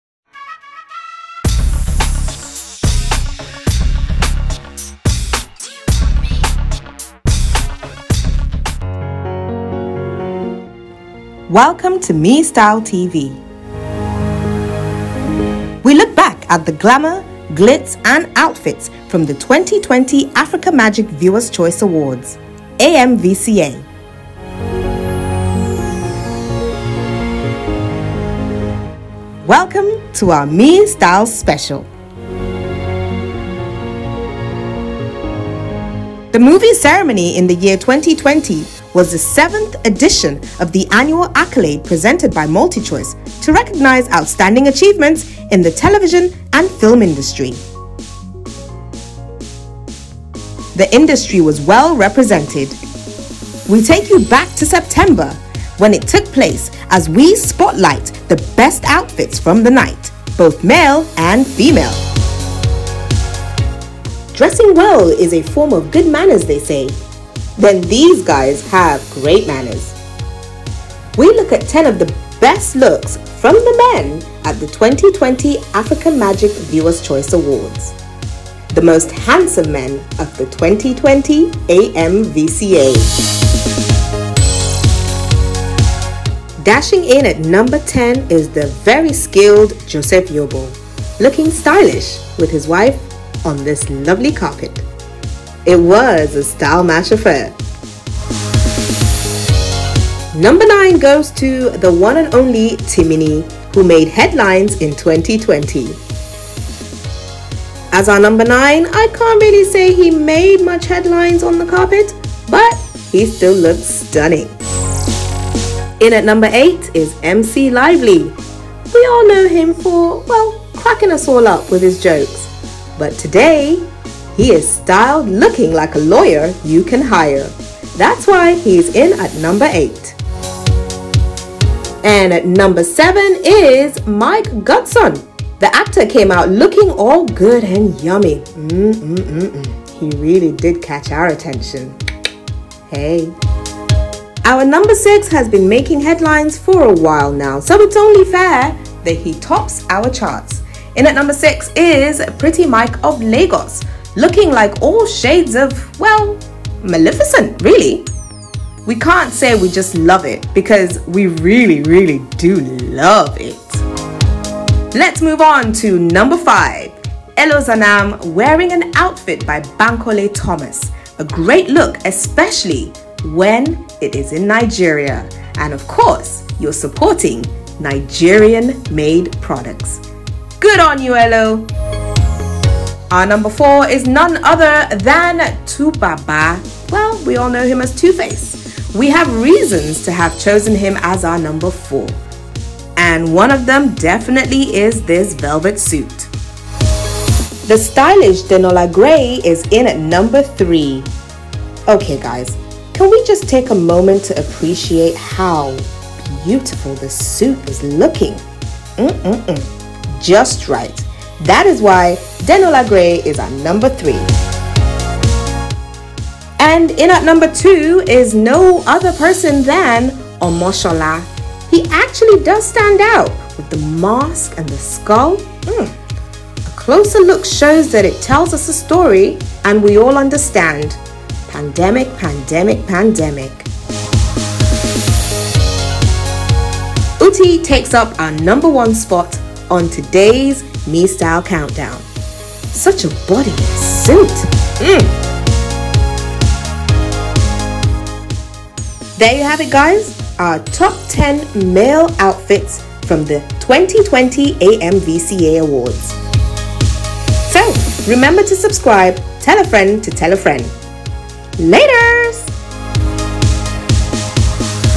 🤵 Joseph Yobo, Timini Egbuson and More Are The Best Dressed Males at 2020 AMVCA | MeStyle TV Special
Joseph Yobo, IK Ogbonna, Timini Egbuson , Pretty Mike, Uti Nwachukwu and more to makes our list of best dressed male from the MeStyle TV's 2020  Best Dressed Female at African Magic Viewers Choice Awards Recap.

Here is the concluding half of the two  - part special of the Fashion extravaganza, courtesy of MeStyle TV

The 2020 African Magic Viewers Choice Awards (AMVCA) 2020 might be over but we are still reminiscence about some fashion statements. The red carpet hosts, Ebuka Obi-Uchendu and Toke Makinwa kicked off the night with fancy and dapper looks. Sharon Ooja looked like a golden goddess in her stunning look. Idia Aisien was a vision in white. Pretty Mike and Chris Okagbue added costumey accessories that had every tongue wagging.

All in all, the seventh AMVCA ceremony delivered exactly what it promised: a celebration of Nollywood and African glamour in all its forms.